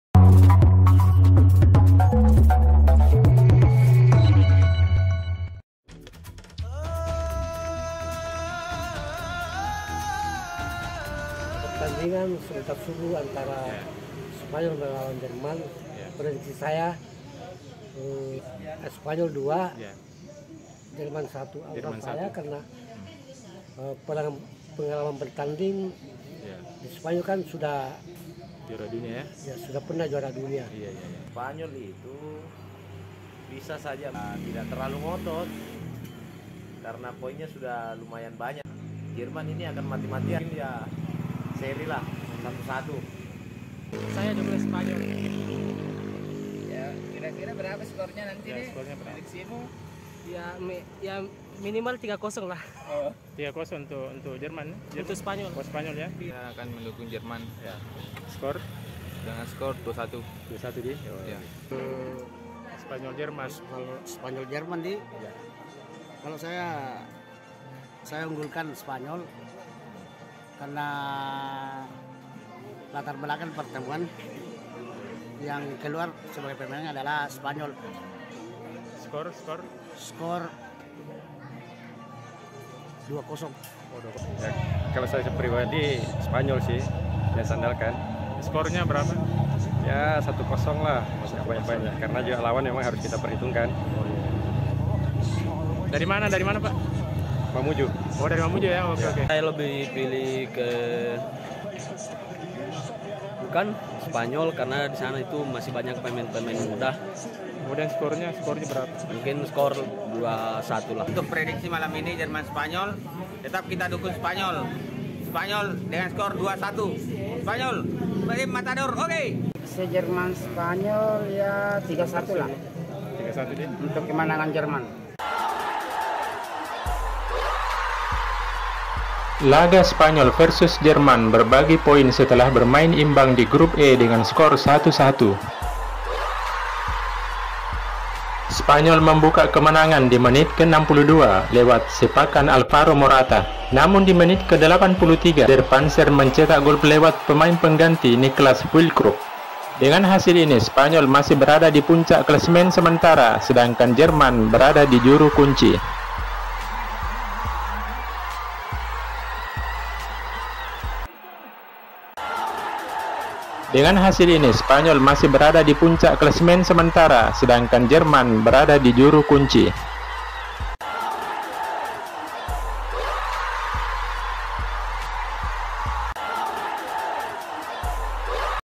JERMAN APES HADAPI SPANYOL ⚽ Prediksi skor & Fakta pertandingan
Laga ke 2 grup bagi timnas Spanyol melawan jerman berakhir imbang 1-1 28 nov 2022 dini hari.